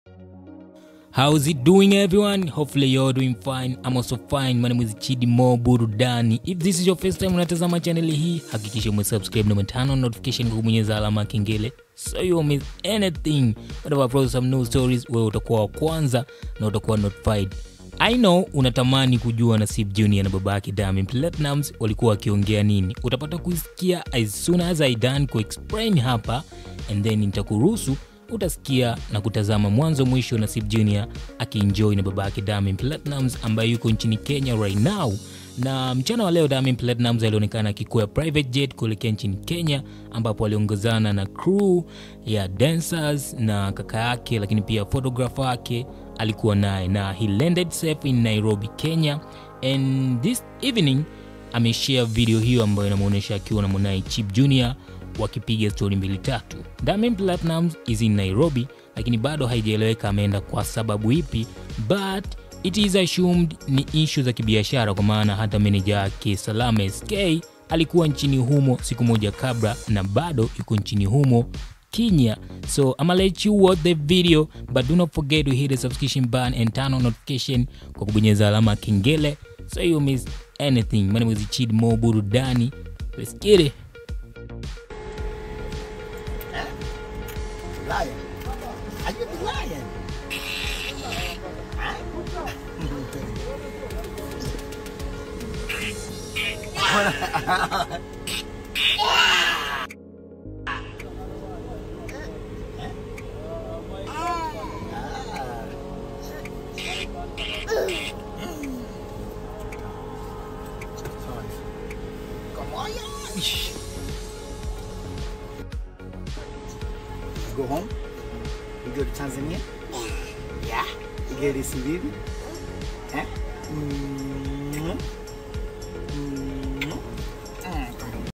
DIAMOND akutana Tena na Mwanae NASEEB JUNIOR nchini KENYA,utapenda Wanavyotaniana,amuita SIMBA mdogo
Video hii inamuonesha Bossi wa WCB Wasafi Diamond Platnumz akiwa na mwanaye Naseeb Junior nchini Kenya